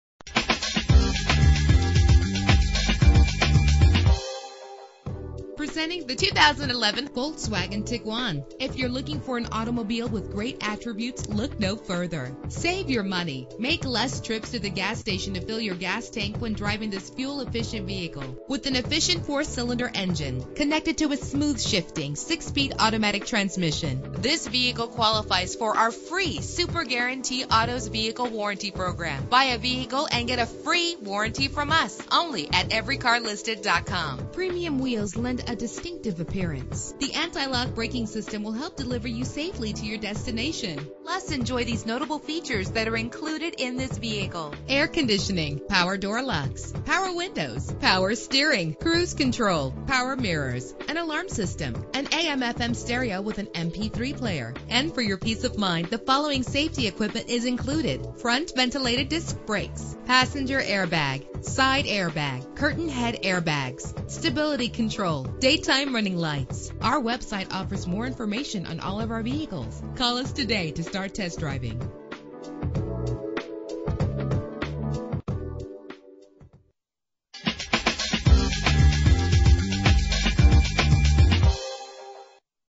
2011 Volkswagen Tiguan - Orland Park IL
Year: 2011
Make: Volkswagen
Model: Tiguan
Trim: S
Engine: 2.0 liter inline 4 cylinder 16 valve
Transmission: 6-Speed Automatic
Color: Black
Mileage: 9
Address:
8920 W 159th St